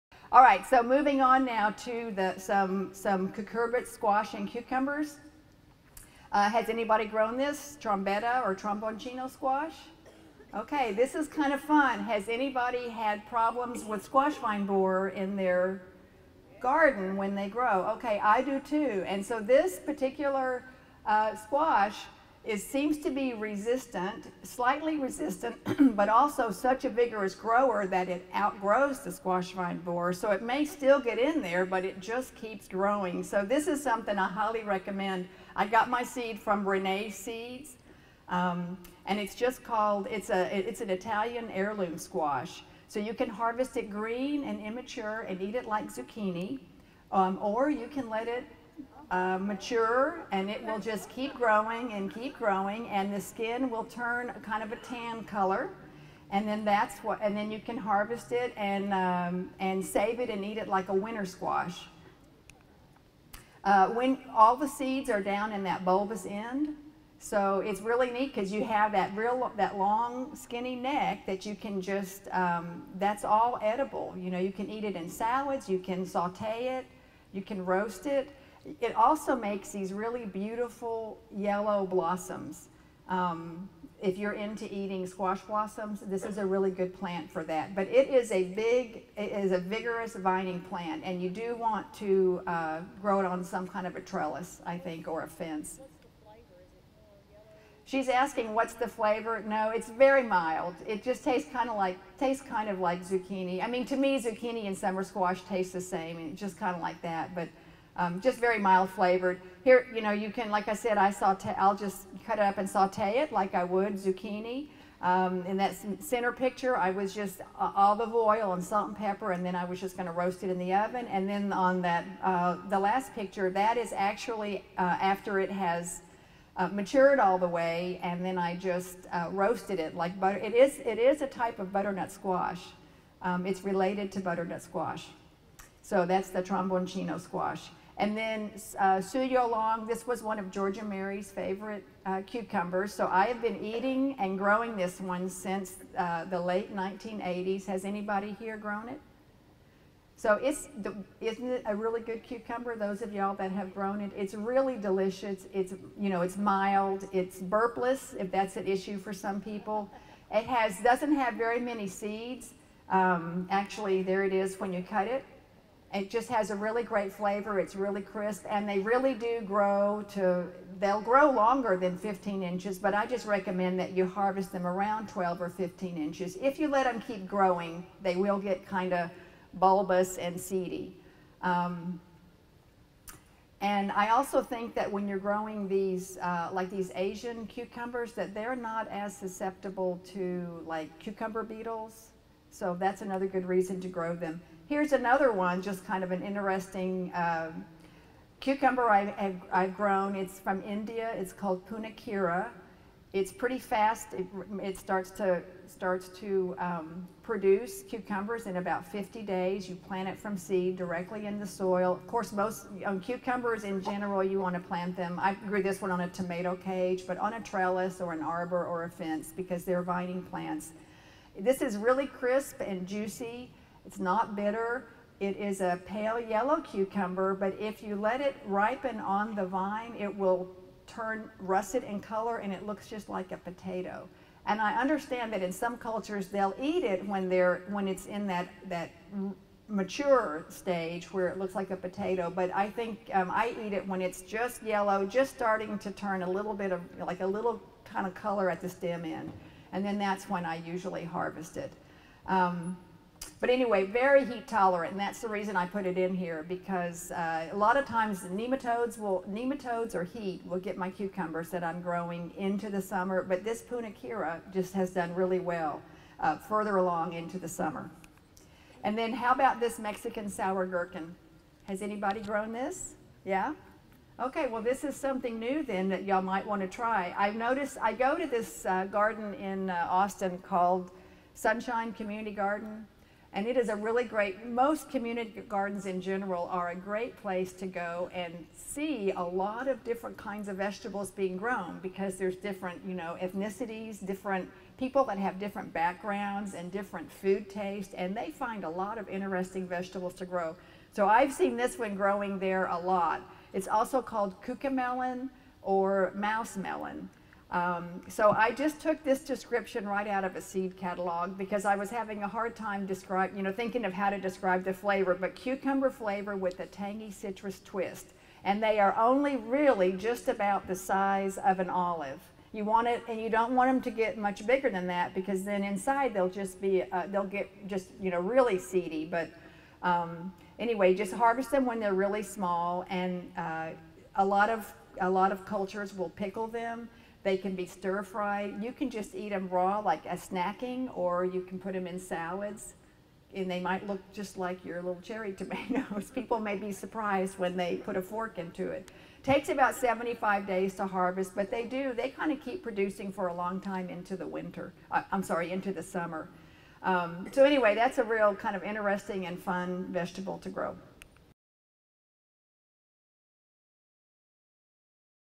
Cucurbita Squash and Cucumbers
This video explains how to grow cucurbita squash and cucumbers, and tells us what the different types you can grow.